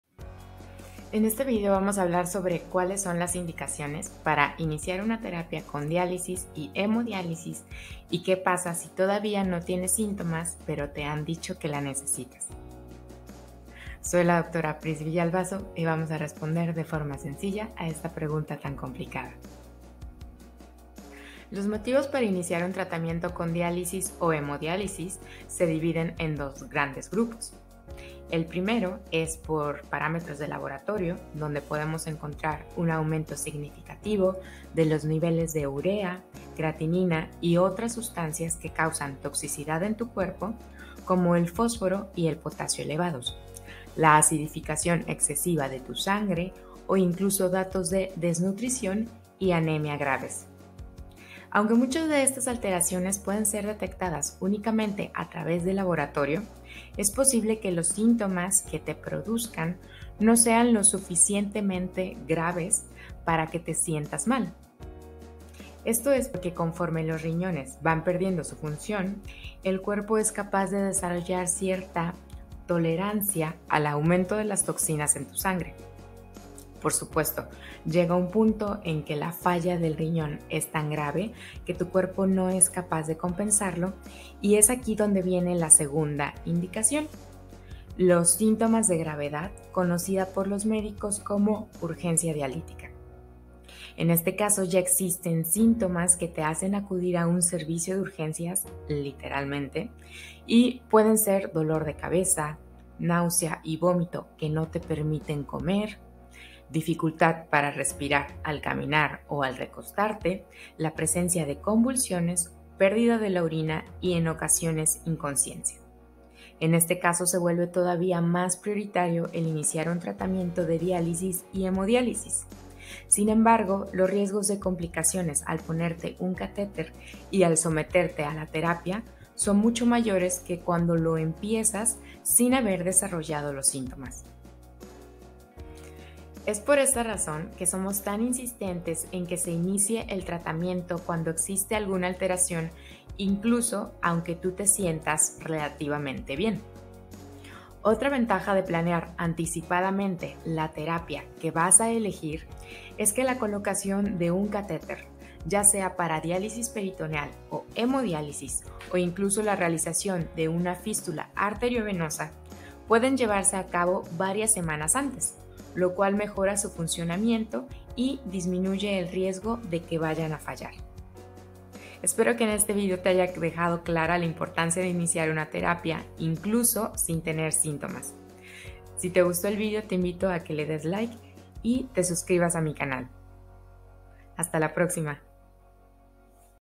Inicio de diálisis (explicación de indicaciones en insuficiencia renal)
Una situación muy común es que los pacientes con insuficiencia renal quieren saber cuándo necesitan dializarse o cómo saber si necesitan dializarse. Muchas veces la persona puede sentirse relativamente bien y no entienden por qué les ha dicho el médico que le van a empezar la diálisis.
En este video te explico las indicaciones más comunes y los motivos por los que proponemos iniciar un tratamiento aún si no tienes síntomas sobre todo las ventajas que puedes tener con el inicio de diálisis.

Si quieres saber más sobre enfermedad renal crónica, y para qué y en qué consisten los distintos tratamientos como diálisis peritoneal o hemodiálisis te invito a que visites mi canal.